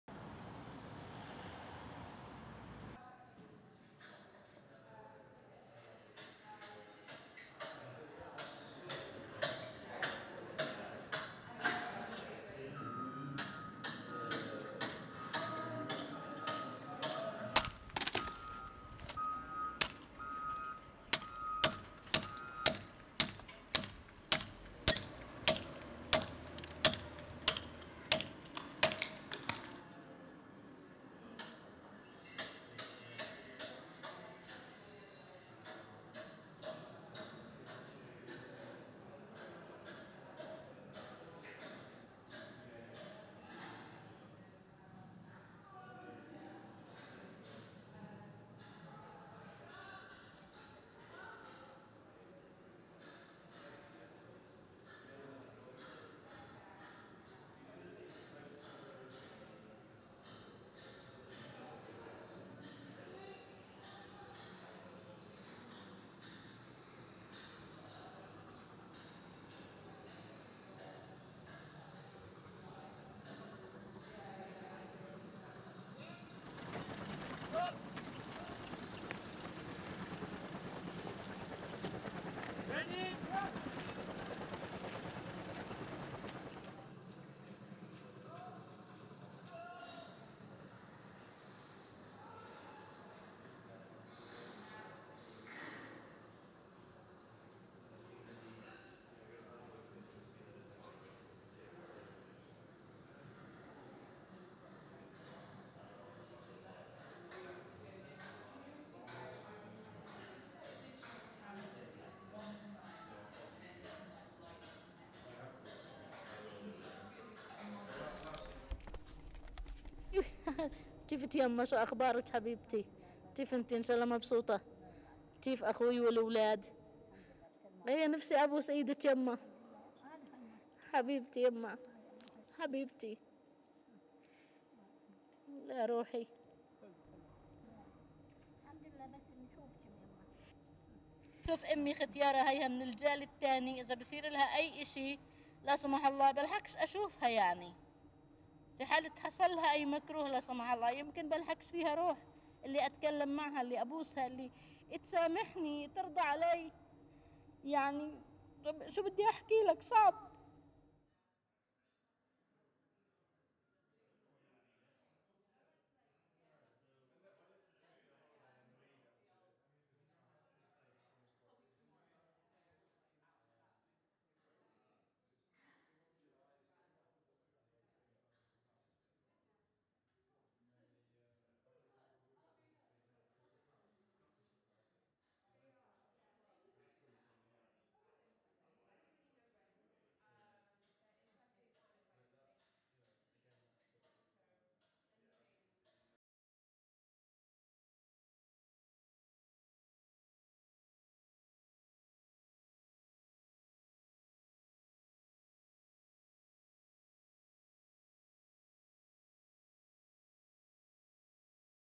Khaled Jarrar Whole in the Wall
Palestinian artist Khaled Jarrar's first UK solo show at Ayyam Gallery London runs from 20 June - 3 August, with an exciting exhibition of new and recent works. His practice incorporates performance, photography, video and sculpture to explore themes of statehood, legitimacy, historical narrative, political authority and freedom. One of Ayyam's most diverse and internationally recognised artists, Jarrar was born and still resides in the occupied West Bank. His works document his observations on life as a Palestinian and reference politics as part of daily life.  His sometimes satirical approach to the absurdities visited on him and fellow citizens of Palestine become the catalyst and the subject of his output, offering us a potent alternative account of life in a contested land.